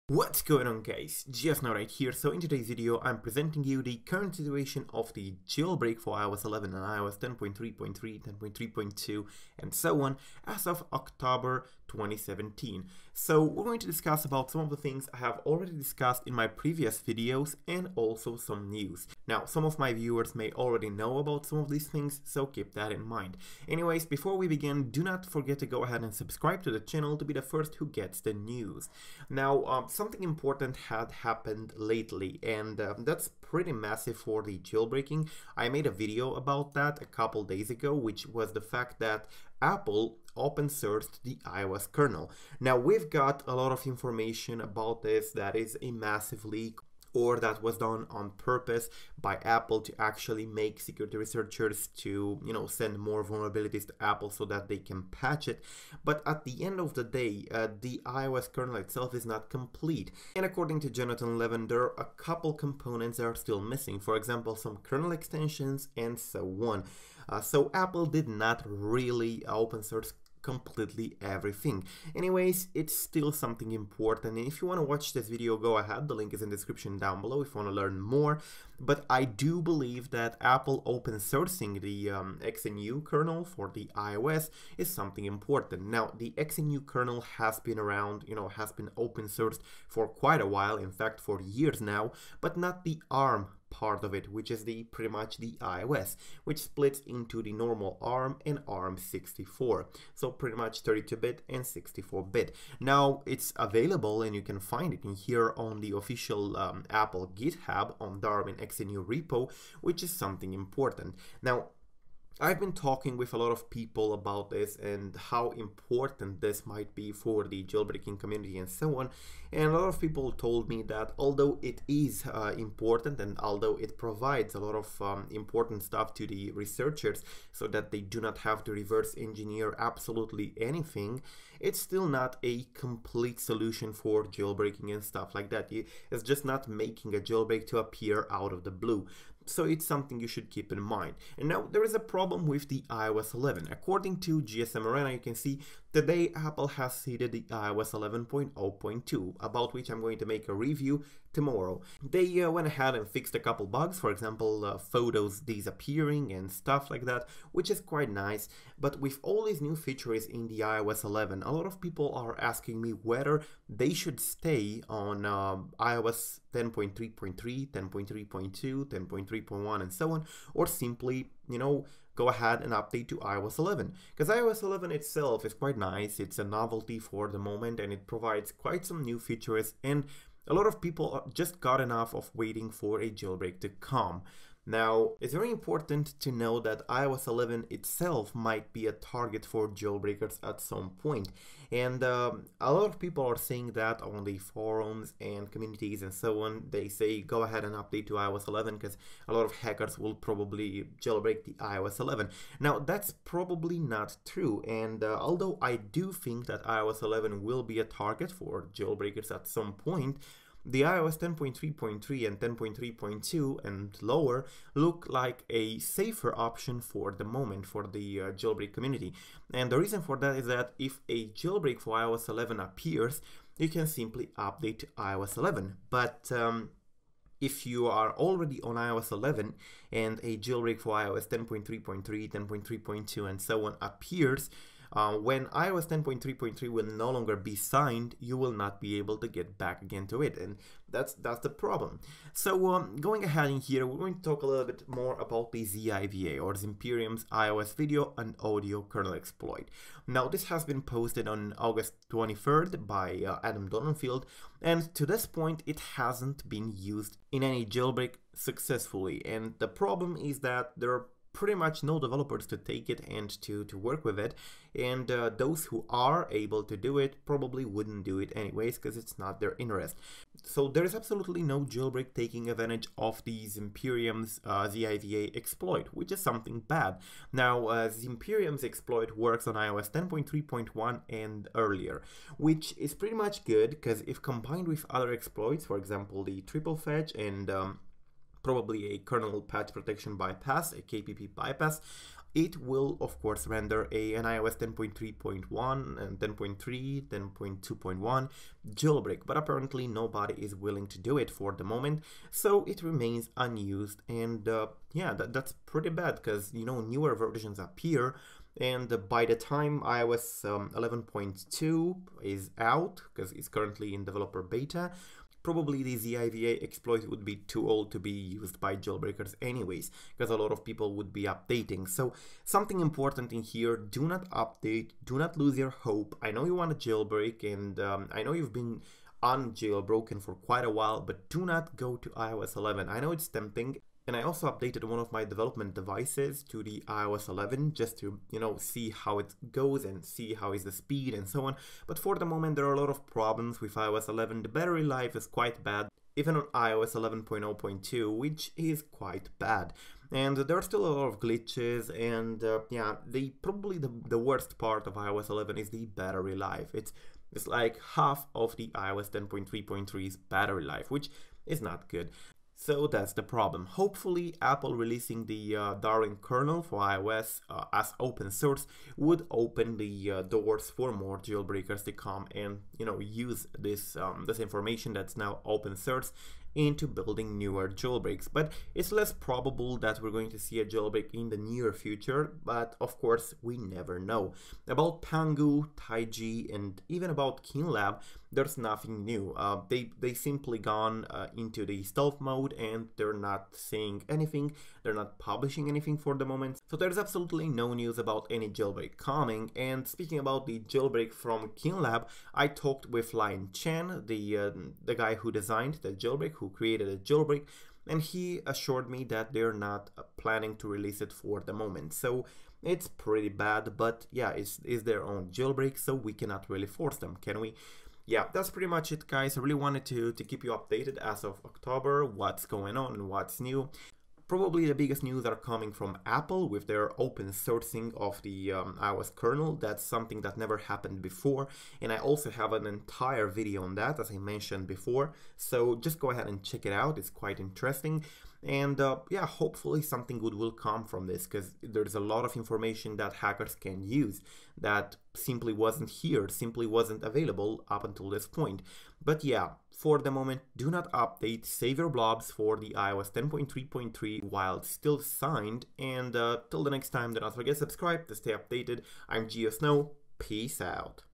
iOS 10.3.3 / iOS 11 JAILBREAK UPDATE & STATUS (OCTOBER WEEK 1)
In today's video we're discussing about the current jailbreak status for iOS 11 and iOS 10.3.3 as of October 2017. We're going to see what is the current situation on the jailbreak scene, and we're going to take a second look at Apple's initiative to Open-Source the XNU Kernel for iOS.

Really, the biggest news that may really influence the jailbreaks in the future is Apple's decision to make the iOS kernel open-source.
XNU kernel has been open-source for years, only not the iOS part of it. With this totally unexpected release, Apple gave jailbreak developers a better perspective over the code of the Kernel and some of the functionality that otherwise should have been reverse engineered in a quite painfully long process.

Although there are some components available that can render at least an iOS 10.2.1 jailbreak currently, nobody seem to be willing to use them. I am talking, of course, about Adam Donenfel'd ziVA Kernel Exploit that remains unused currently, about triple_fetch, again unused, which combined with yalu102's KPP bypass, can easily produce a new iOS 10.2.1 jailbreak.

The jailbreak community is pretty quiet for the moment, with the release of iOS 11, and now iOS 11.0.2, a lot of people are asking me whether they should update or not. iOS 11 is the current novelty, and although it does provide some great new features and revamps, it is still not recommended to update, especially if you want to jailbreak, but even if you don't, the battery life on iOS 11 is terrible.